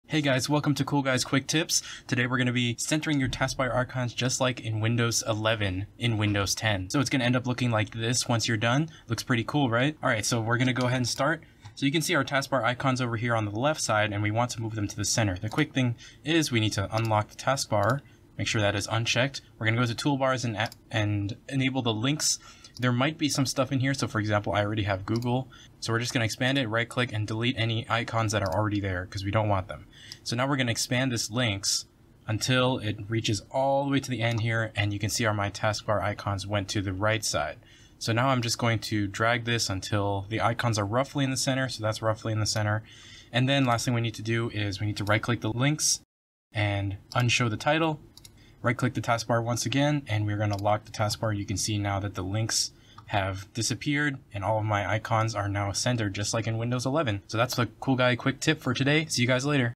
Windows 11 Taskbar in Windows 10!
Want a taste of the new Windows 11 UI but can't or don't want to run it on your own computer? Center your taskbar icons in Windows 10, just like in Windows 11!